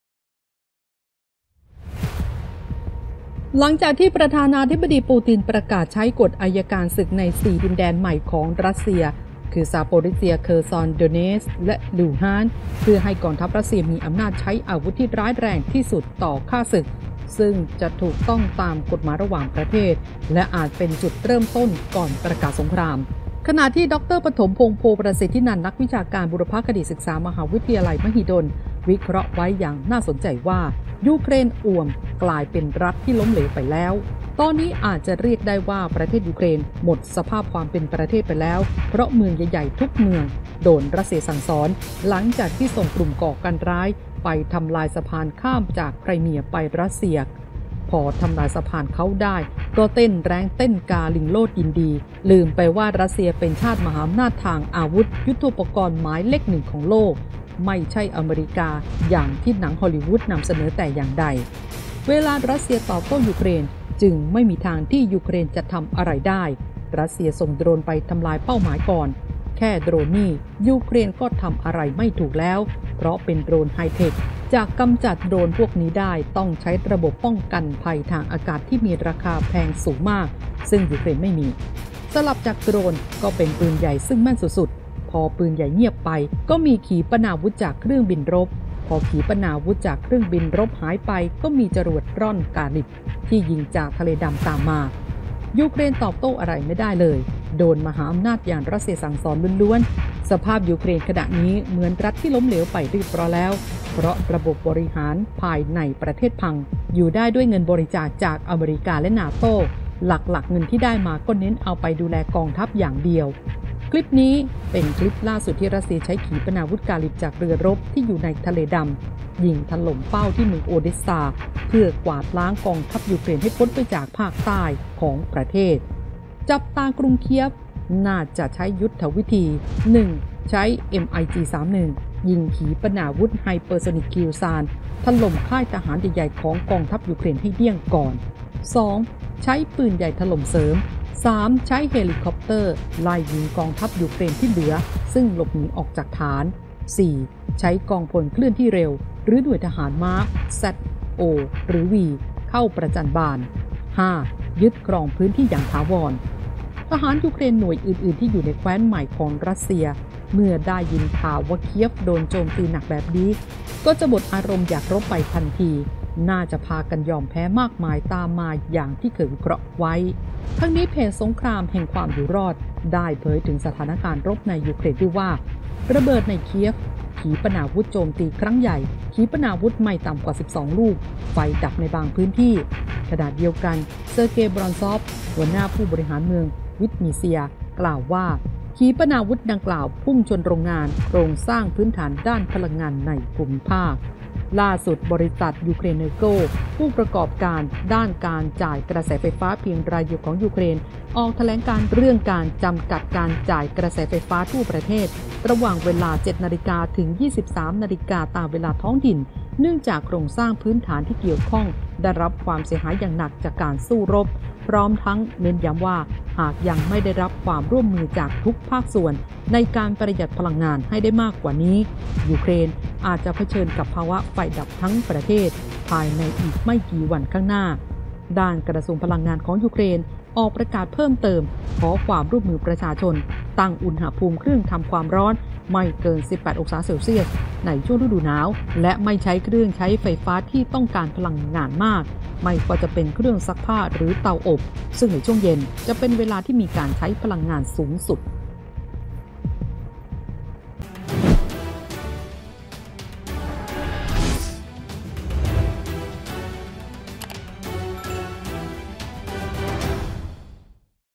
ลิงโลดไม่กี่วัน เจอ"รัสเซีย"สั่งสอน"ยูเครน"อ่วมตอบโต้ไม่ไหว | ข่าวเด่น | TOP NEWS
"หมดสภาพ"กลายเป็นรัฐล้มเหลว "ยูเครน"หัวหมุนทำอะไรไม่ถูก เจอ"รัสเซีย"มหาอำนาจอาวุธ ถล่ม สั่งสอนหนัก ทั้งโดรน ปืนใหญ่ ขีปนาวุธจากเครื่องบินรบ จรวดร่อนกาลิบร์  จับตา"กรุงเคียฟ"  ค่ายทหารใหญ่ ๆ เจอโจมตีเดี้ยงก่อน แล้วส่งหน่วยรบประจัญบาน ยึกพื้นที่ถาวร